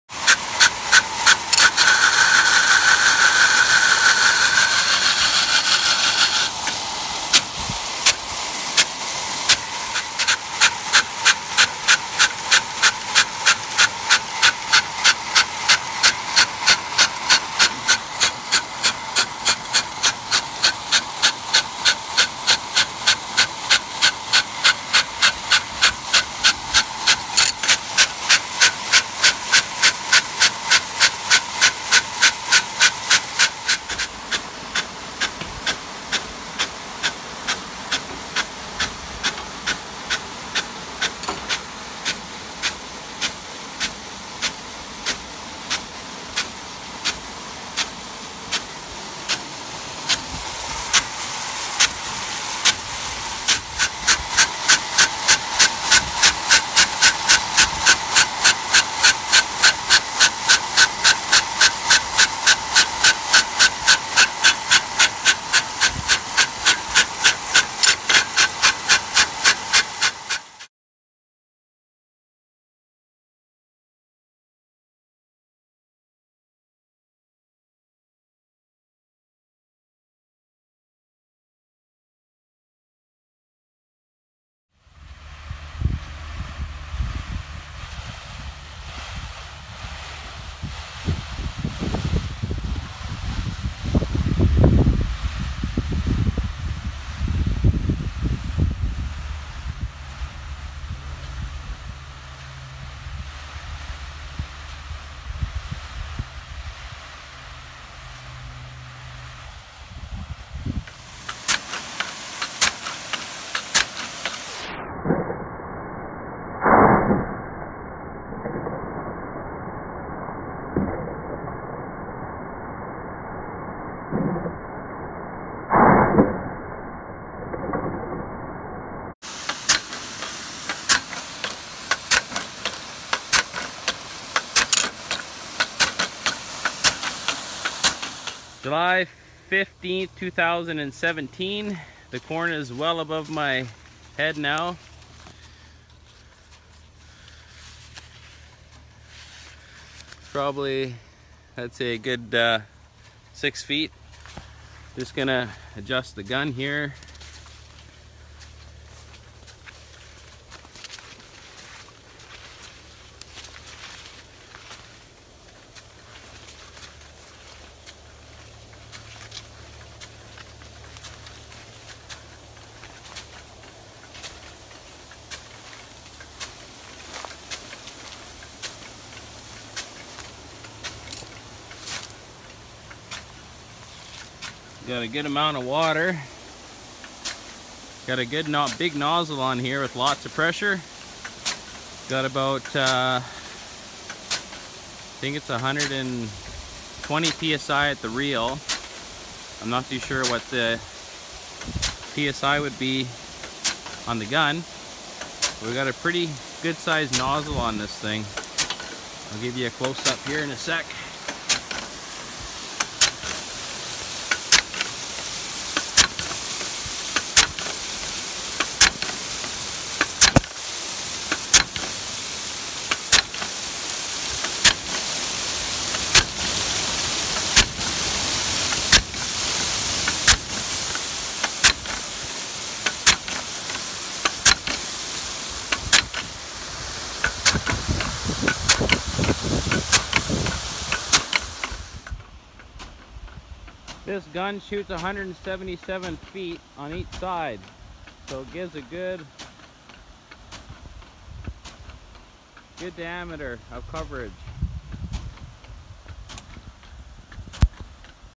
Nelson Irrigation SR150 Not Working
Replaced arm assembly with bearings